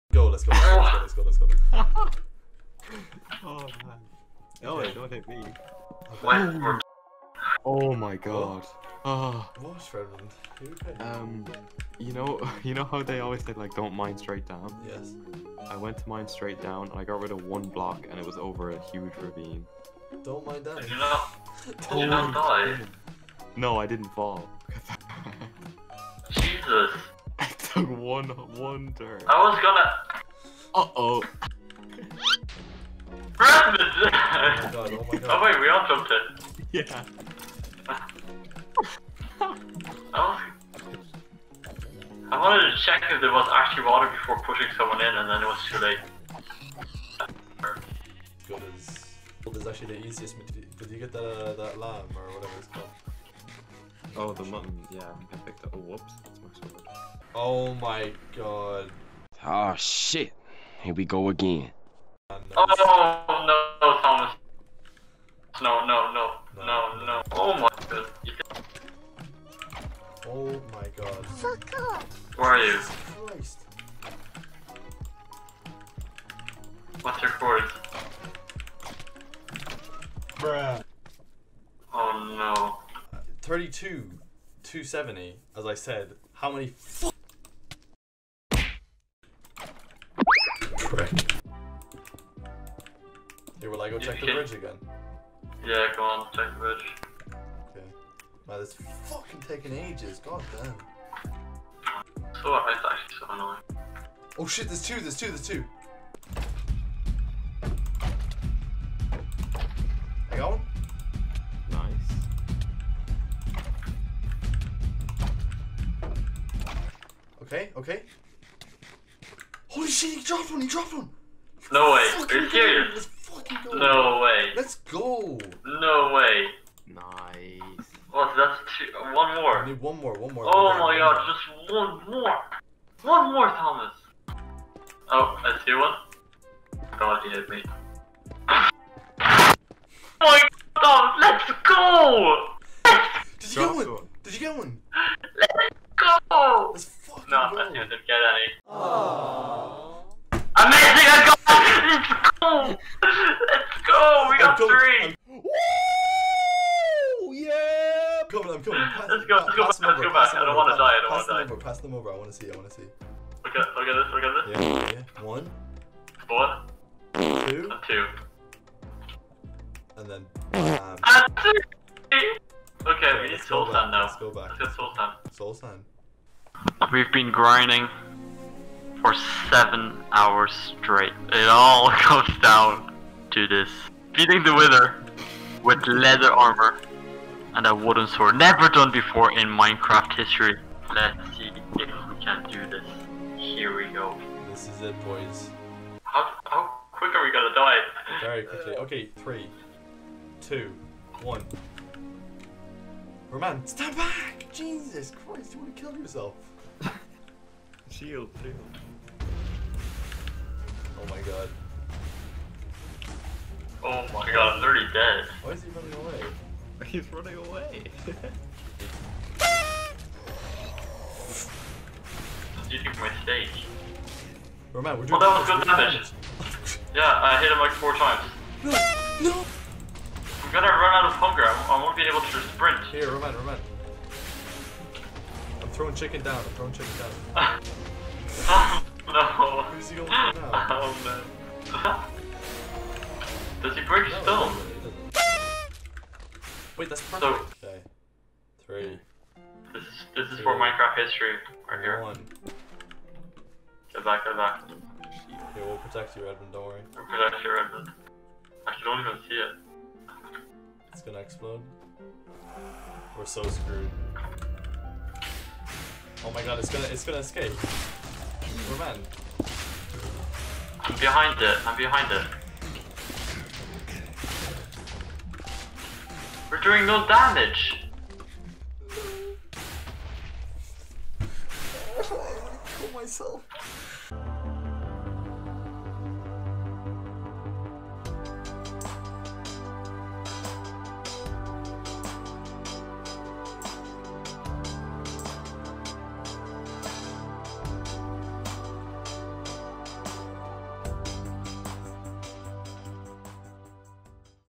How Not To Beat The Wither
The second episode of the How Not To series is here but this time we show you how not to beat the wither. Enjoy!